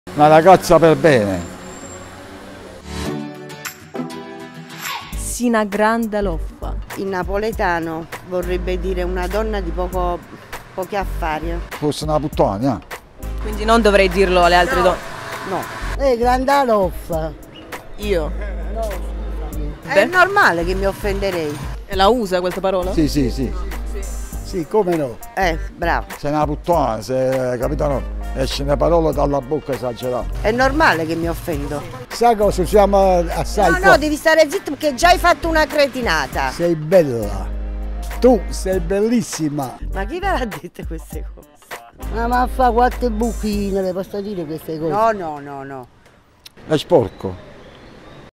Chi è una LOFFA? MaleParole NAPOLETANE 8
Cosa vuol dire LOFFA? Rispondono i Napoletani!
Per questa lezione mi soni persino finta STRANIERA!
MaleParole lezione 8
Corso di napoletano scostumato con la Ragazza Dell'Est... di Napoli.
😊 MANDATE QUESTO VIDEO AI VOSTRI AMICI DEL NORD!